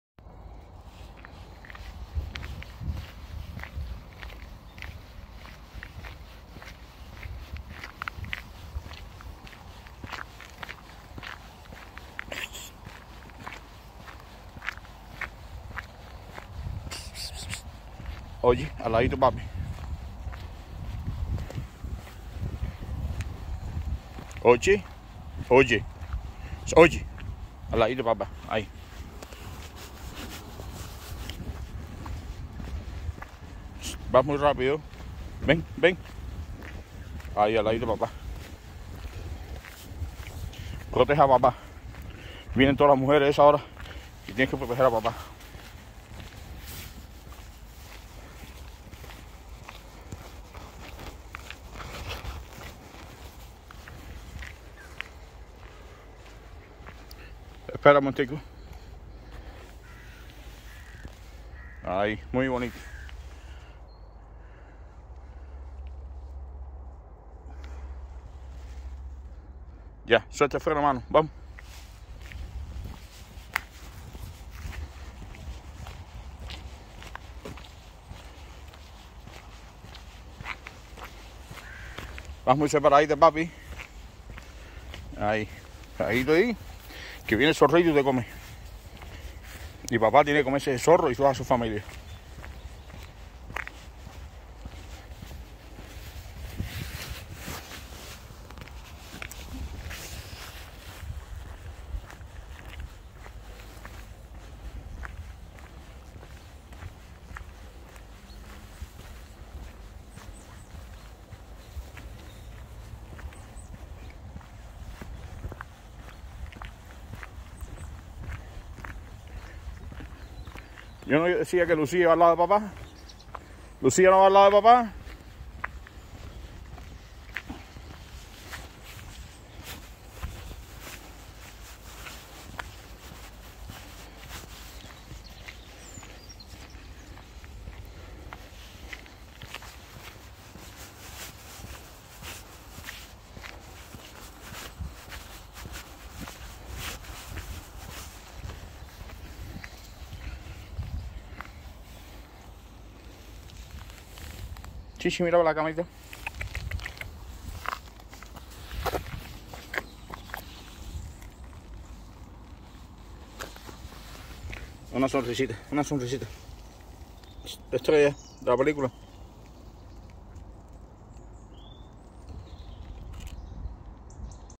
Lucía María the Chihuahua debe caminar junto a su papá que hay zorros y ellos son peligrosos.
Lucía María the Chihuahua debe caminar junto a su papá que hay zorros y ellos son peligrosos para una perrita tan pequeñita.  En realidad los zorros no se acercan a las personas, pero hay que evitar los peligros.